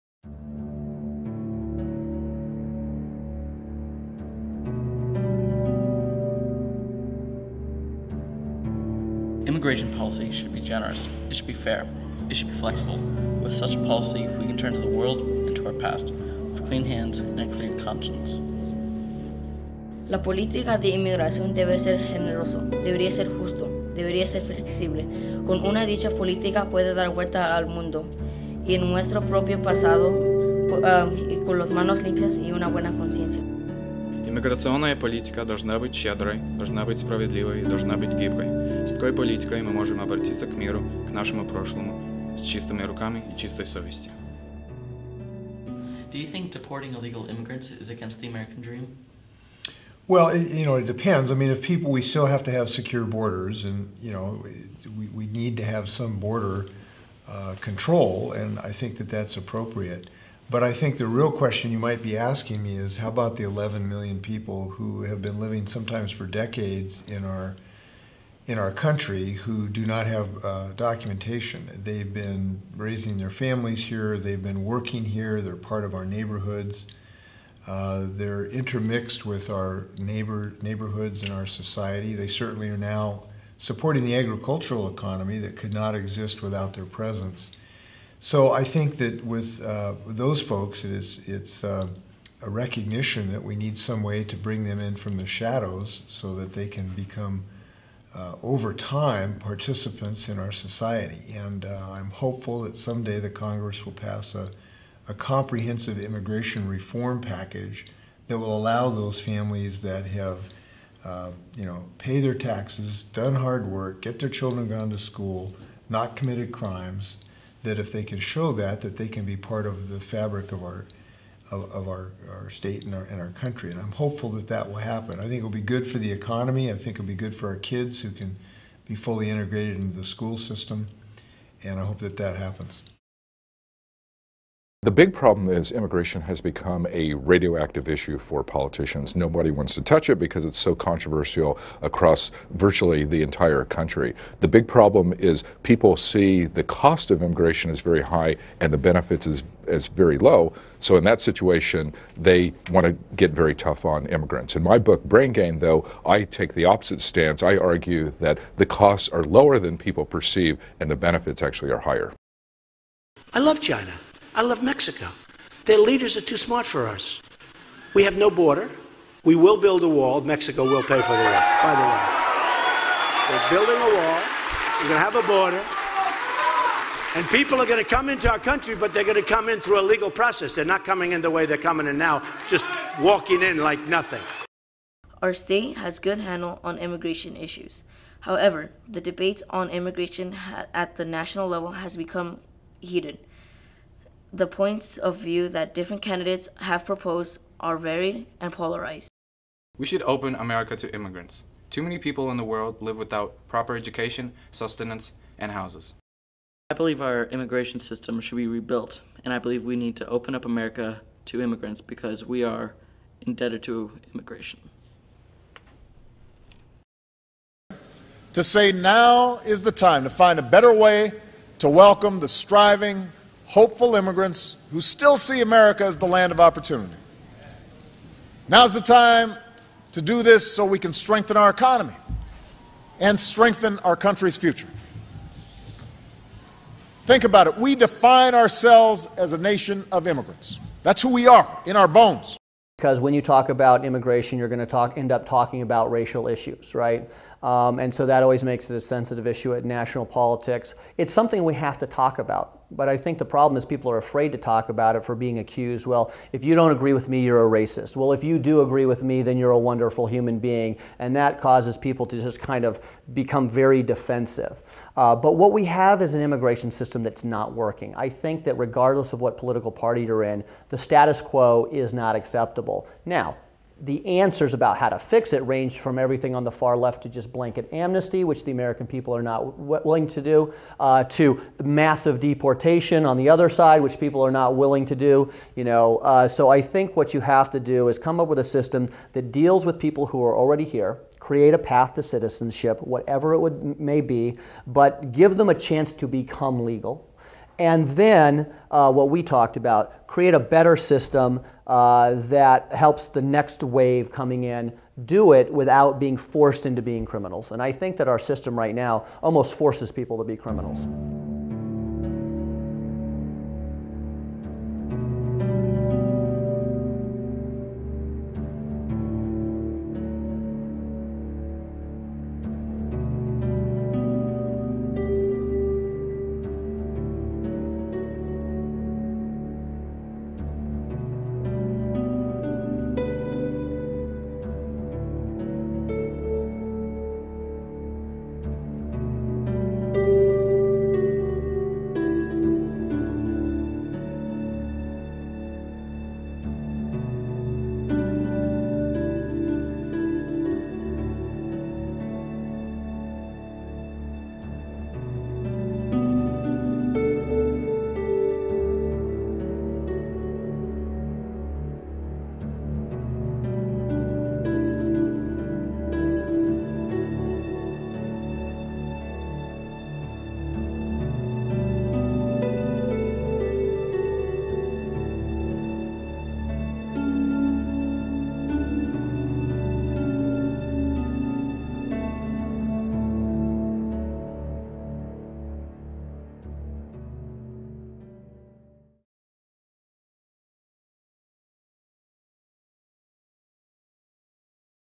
Indebted to Immigration
Indebted to Immigration: Jake Chambers, Edgar Mejia-Cruz, David Beletskiy interview Gov. Jay Inslee (D) and representative Matt Manweller (R) about immigration issues that exist locally and nationally. C-Span video Student Cam Competition